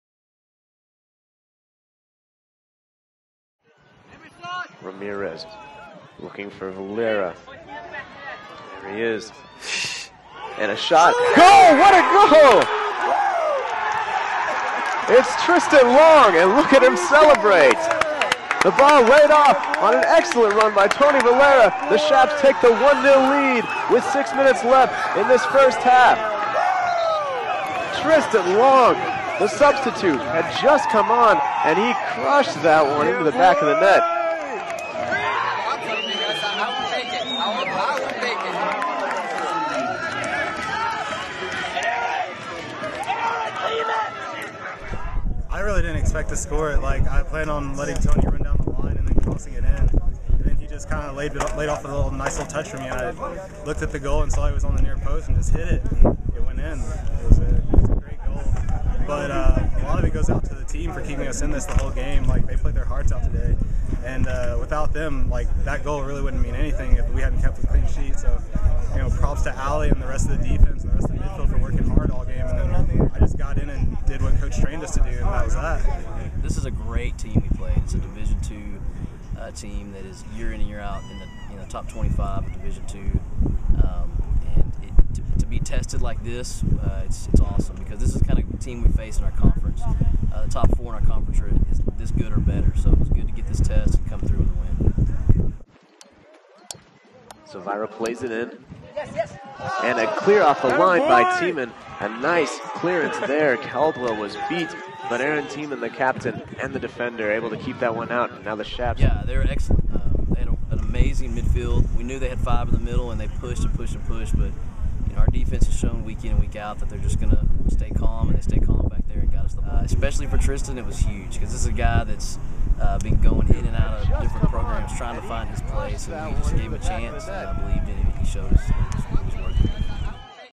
LCU Men's Soccer Recap vs. ENMU (9/7/10)
Chaps defeat ENMU 1-0